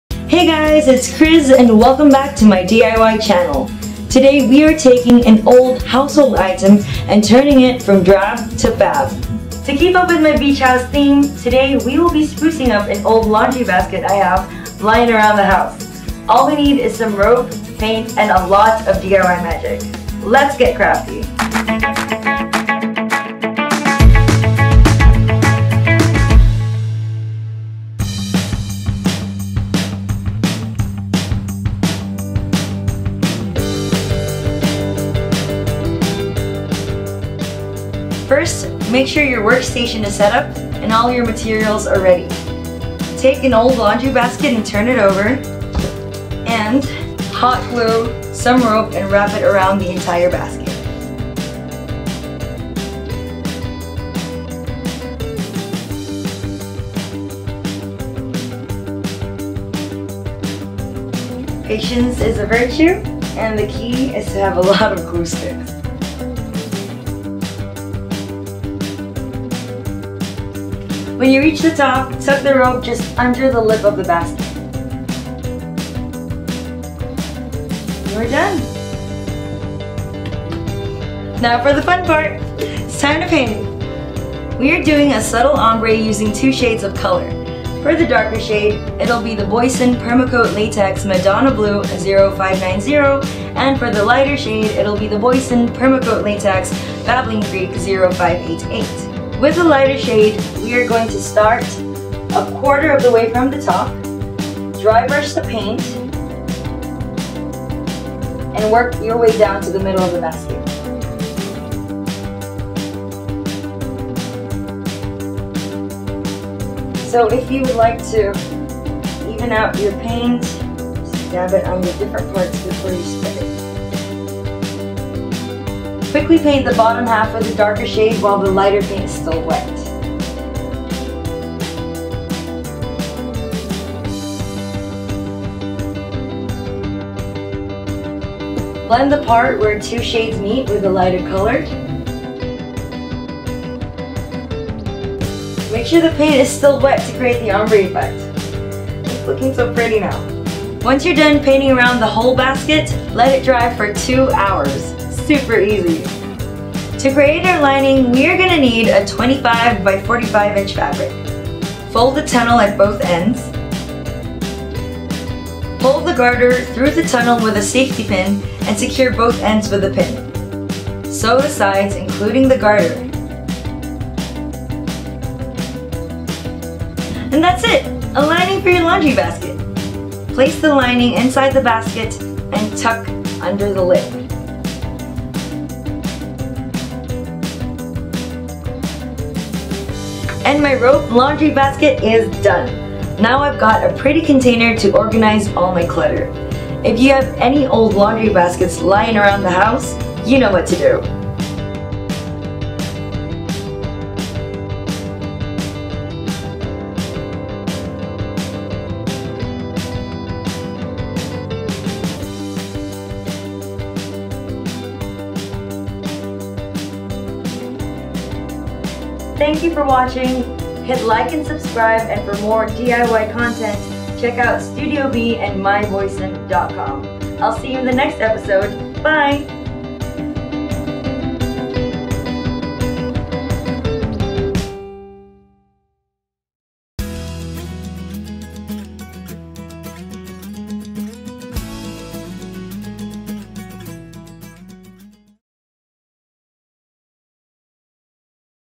Krafty Kriz: How to Make a Rope Laundry Basket
Score a fab storage basket that looks pricey by recycling an old plastic basket with rope and paint.